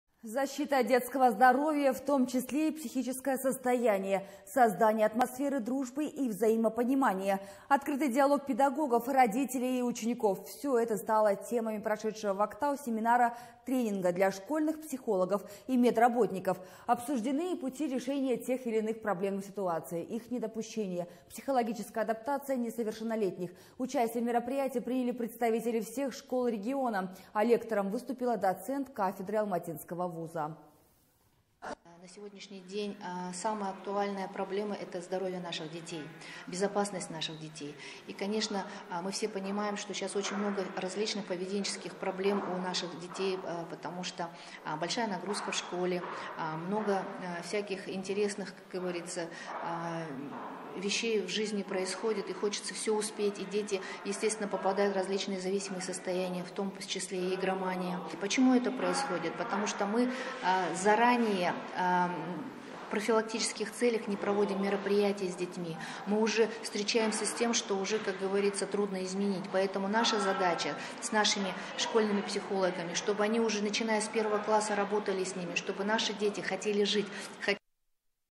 Семинар для школьных психологов и медработников
Защита детского здоровья, в том числе и психическое состояние. Создание атмосферы дружбы и взаимопонимания. Открытый диалог педагогов, родителей и учеников. Все это стало темами прошедшего в Актау семинара-тренинга для школьных психологов и медицинских работников.